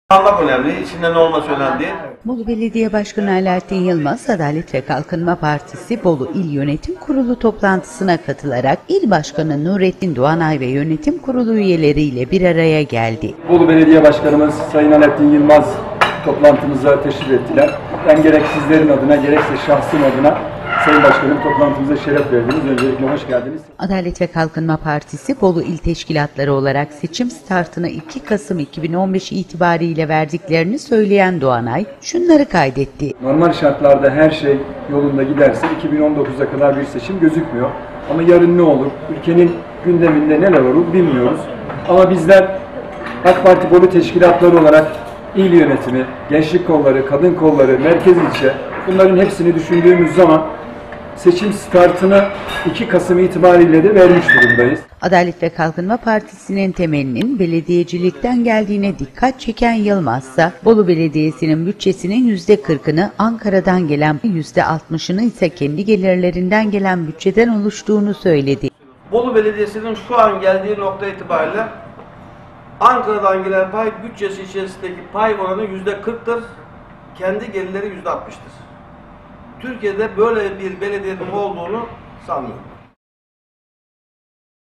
YILMAZ, YÖNETİM KURULU ÜYELERİYLE BİRAYA GELDİ (09.03.2016-BOLU)
Bolu Belediye Başkanı Alaaddin Yılmaz, AK Parti Bolu İl Yönetim Kurulu toplantısına katılarak, İl Başkanı Nurettin Doğanay ve yönetim kurulu üyeleri ile bir araya geldi.